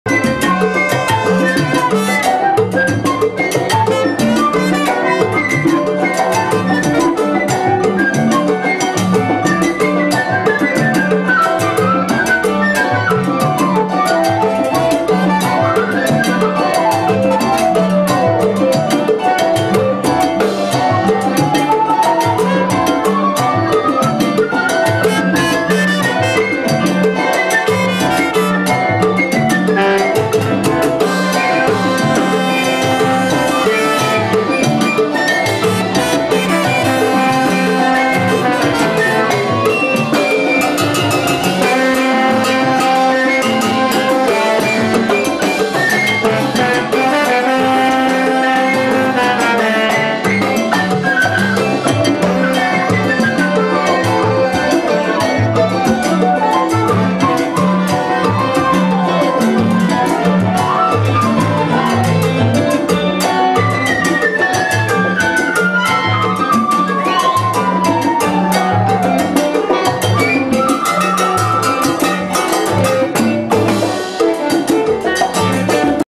With Angel Lebron featuring Art Webb
Playing my White Ampeg Baby Bass(original pickup plus a Piezo pickup mounted onto a wooden bridge) with a 2 channels K&K preamp (to blend the 2 pickups into 1),then a send it to my GK 100RB/ 2x12 Schroeder cab